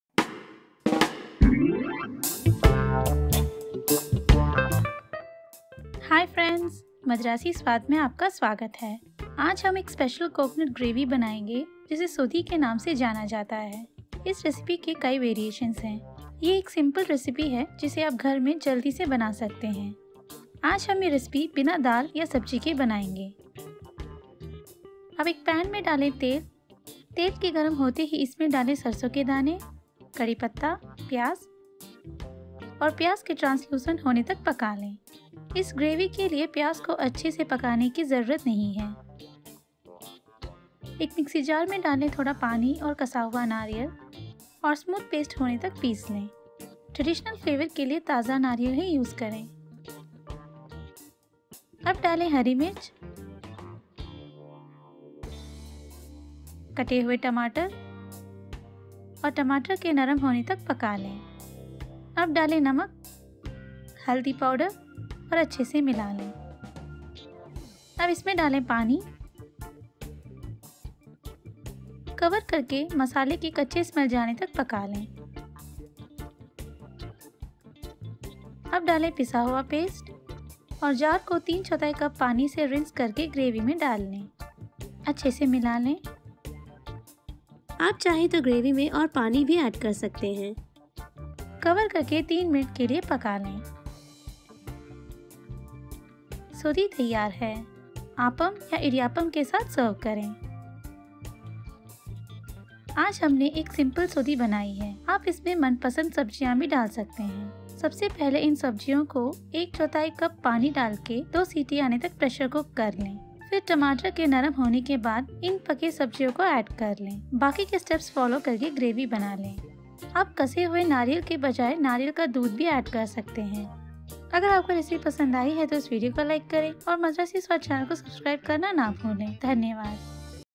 Coconut Gravy Recipe in Hindi / नारियल ग्रेवी / How to make Coconut Gravy at home
In this video, we will see how to make Coconut Gravy (कोकोनट ग्रेवी) in Hindi. This gravy is also known as "Sodhi" in Southern Tamilnadu. There are many variations of this recipe. The recipe varies from family to family. I learned this simplified version from my mom which can be made quickly at home. We will be making it without any dal or vegetables. If you want, you can also add mixed vegetables like Potato, Green beans, Carrot, Green peas etc.

Ingredients:
Curry leaves
Water - 1/2 cup + 3/4 cup (to add at last)

Please try this recipe and share your feedback in the comments below. Keep Cooking! Keep Rocking!!